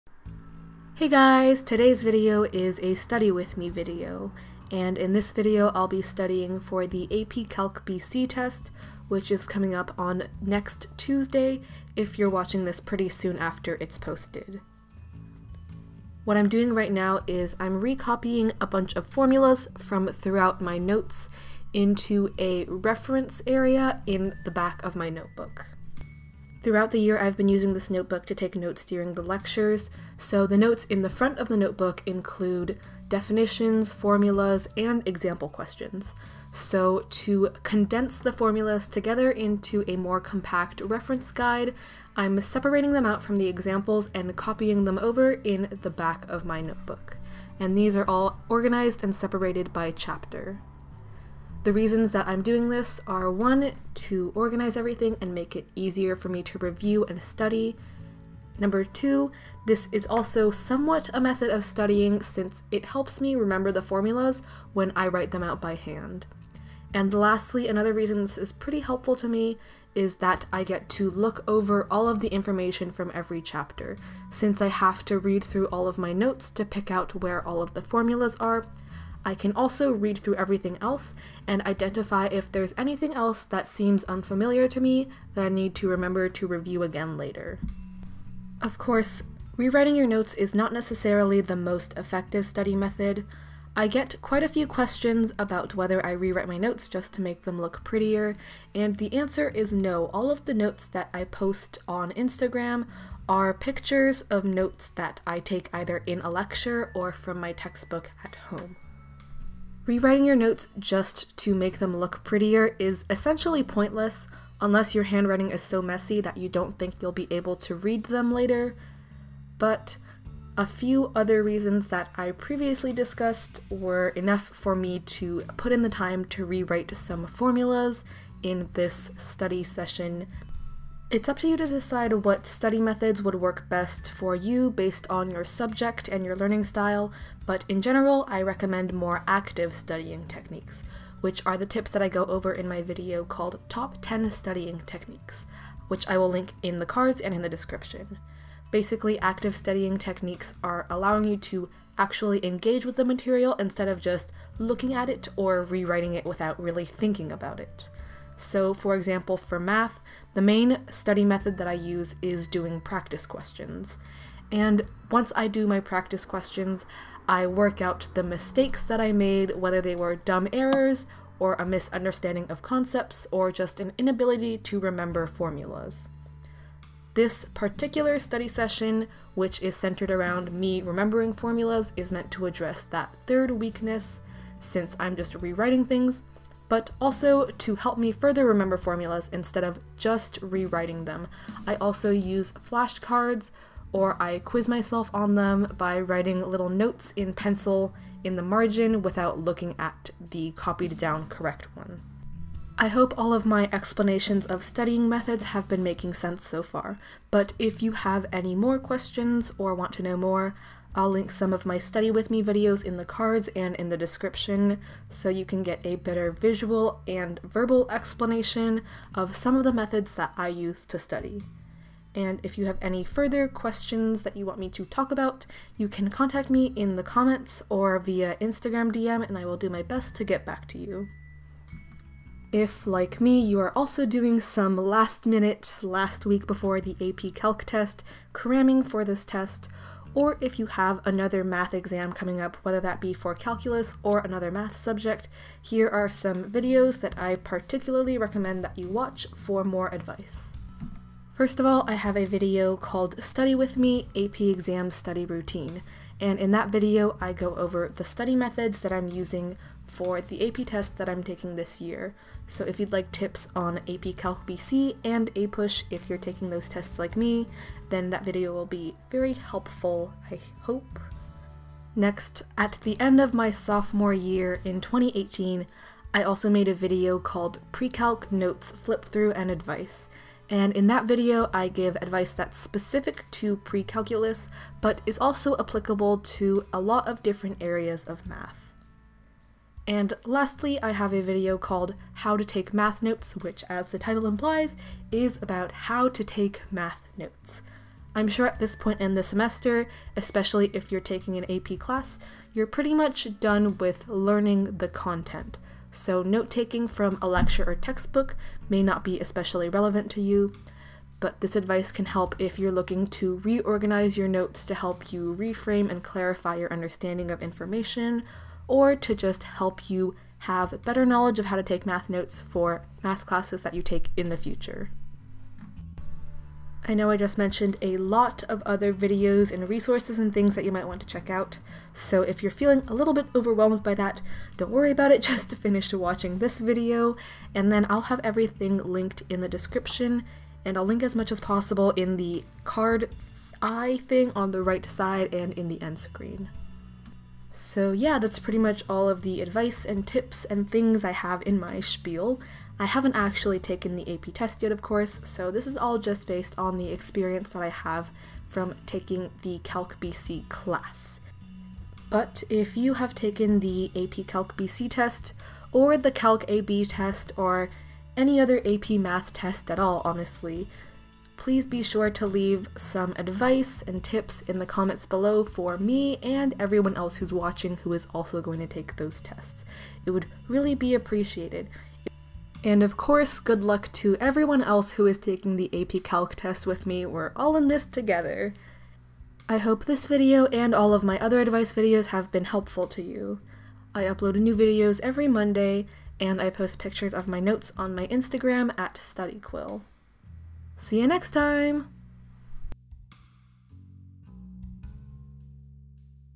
how i take calculus notes 📕 a last minute study with me
Good luck with AP tests everyone!

Supplies used
⋅ Notebook - Kokuyo Campus grid notebook
⋅ Pen (body) - Uni Jetstream Alpha Gel

!! Stationery ≠ studying, these are just listed for my fellow pen enthusiasts! You don't need certain supplies to succeed.